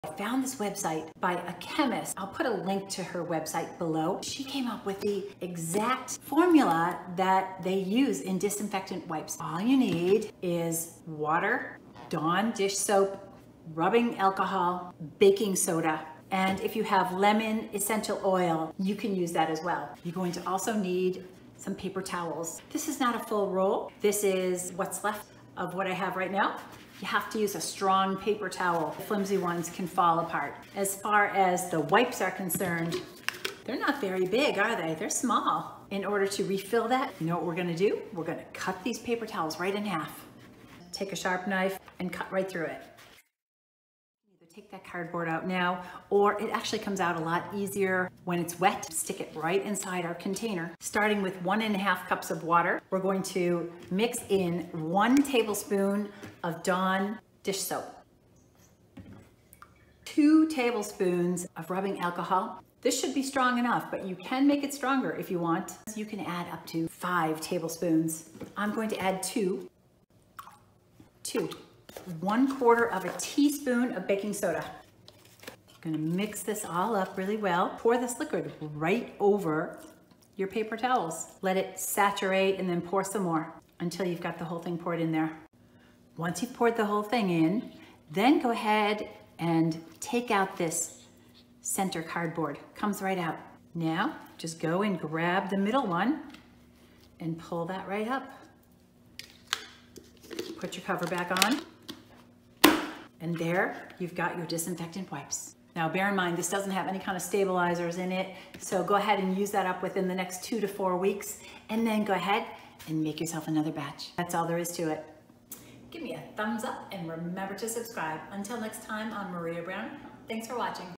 Disinfectant Wipes: The Real Ingredients to make your own (Please Share)
Bleach is NOT an ingredient in disinfectant wipes! Make your own with the real ingredients they actually use in disinfectant wipes! (Based on research from a chemist.)

Check out the chemist's website, it's loaded with good stuff, including this recipe